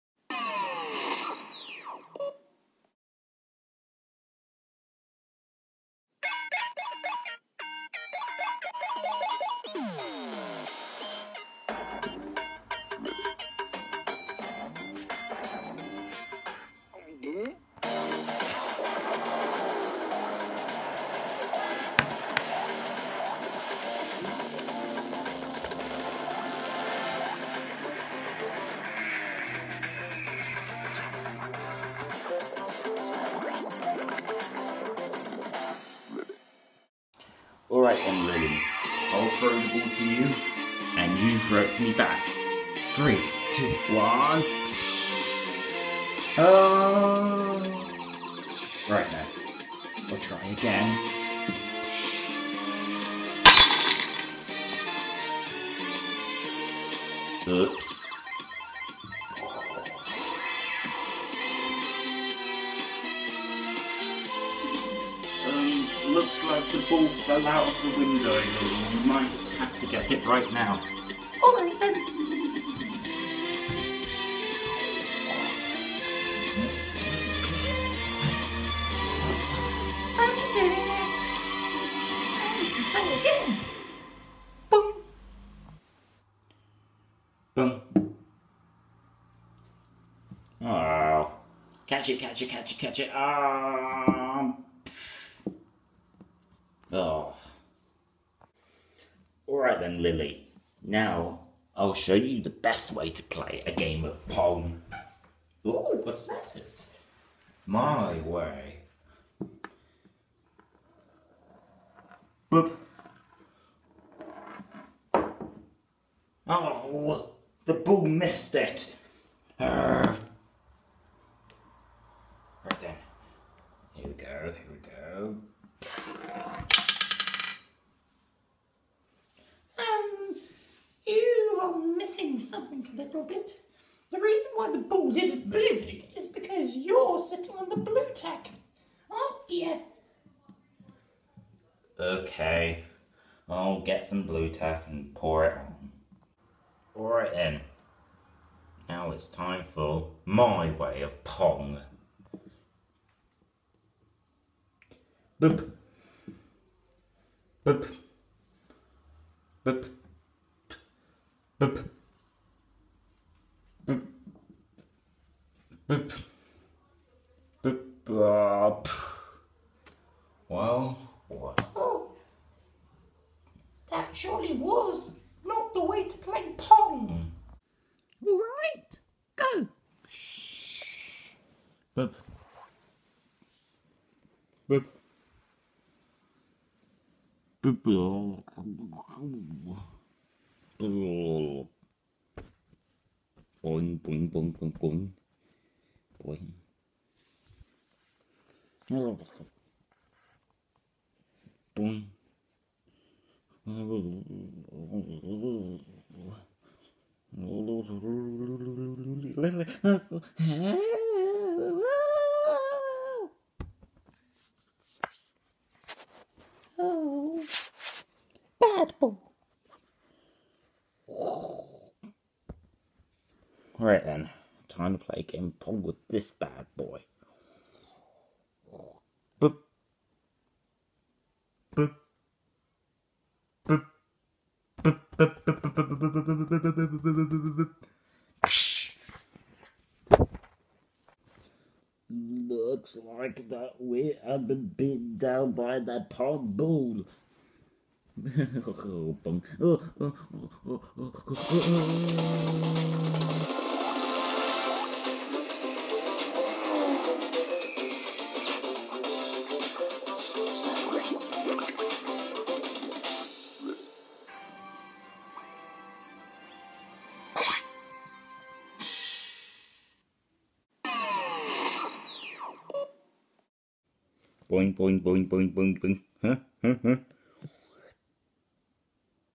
The World goes Pong - Frogger Beyond (Episode 2)
Frogger tries playing a game of Pong with Lillie, but with poor results.